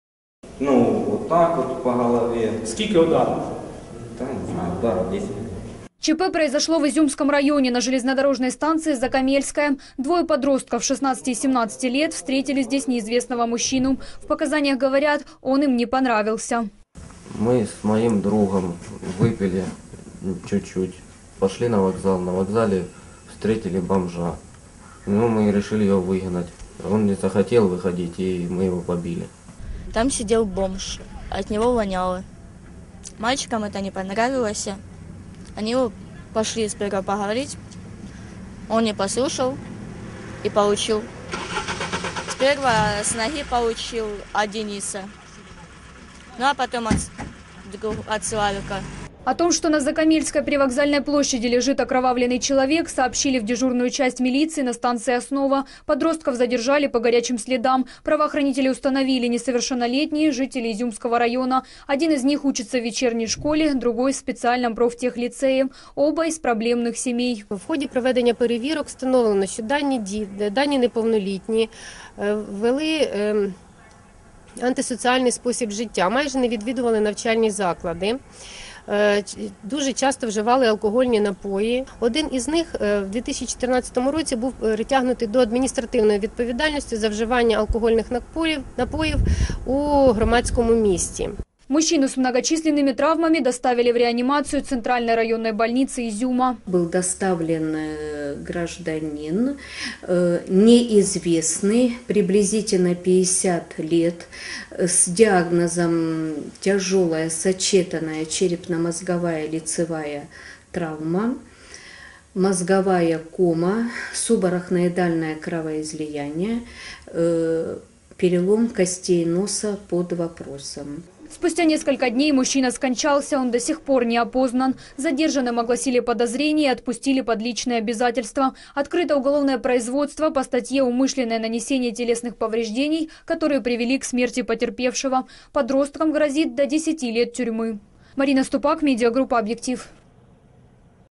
На Харьковщине двое несовершеннолетних убили мужчину
- Преступление руками несовершеннолетних. Двое жителей Изюмщины до смерти избили бездомного - В дежурную часть милиции на станции Основа поступила информация о том, что недалеко от станции Закомельская Изюмского района лежит мужчина с многочисленными травмами. Пострадавшим оказался бездомный мужчина